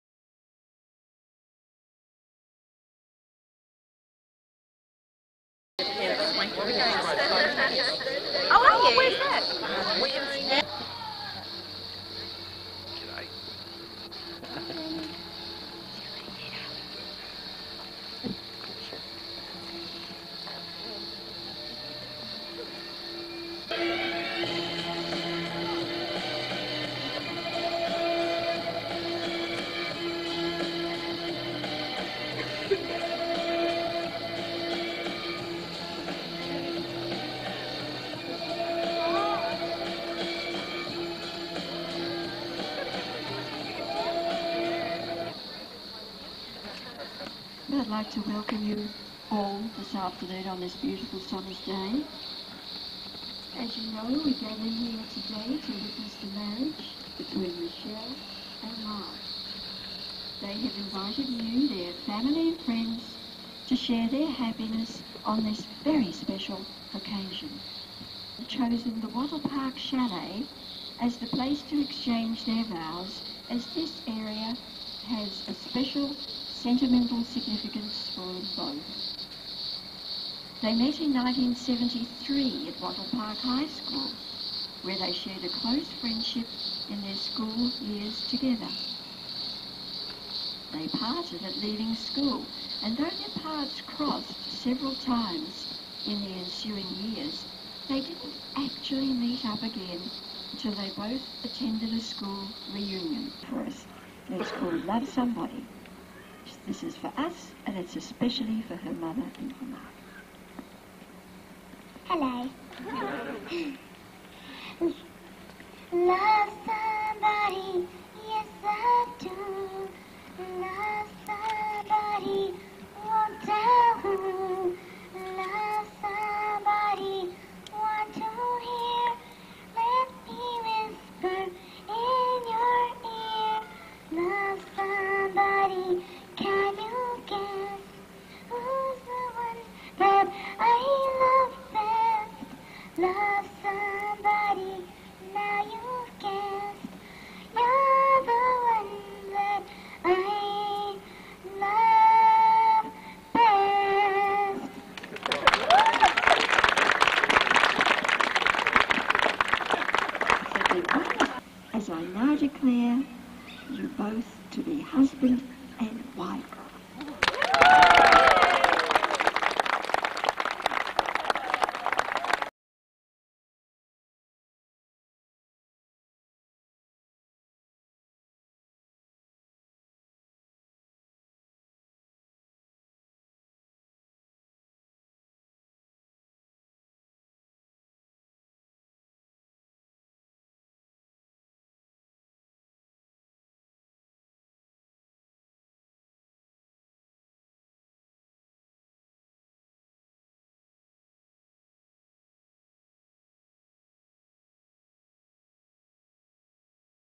Mark and Michelle's Wedding
Some highlights from our wedding 20 year ago!